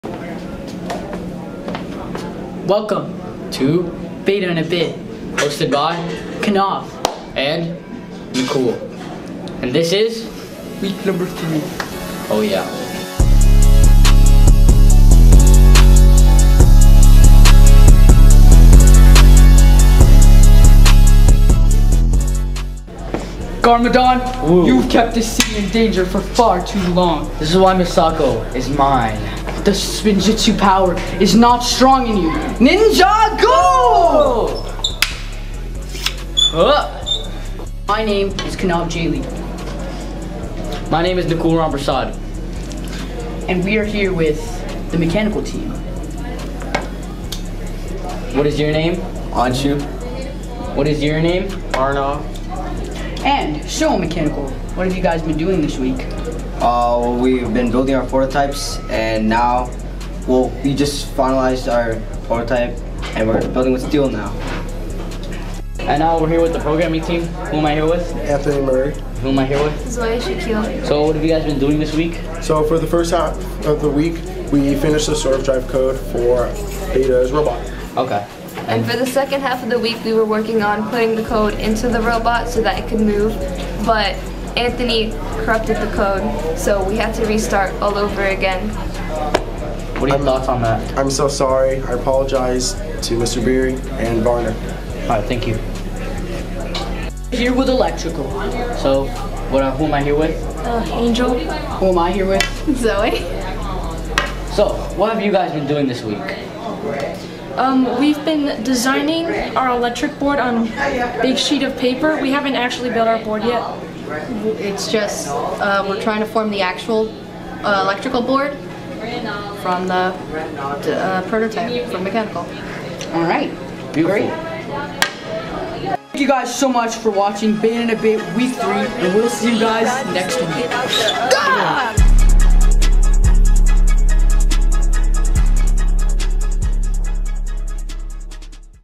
BETA in a BIT 2024: Week 3
Welcome to BETA in a BIT Week 3! In this episode, BETA's subteams discuss what they did in the third week of Build Season!

Filmed by: Jayden Nguyen
Edited by: Jayden Nguyen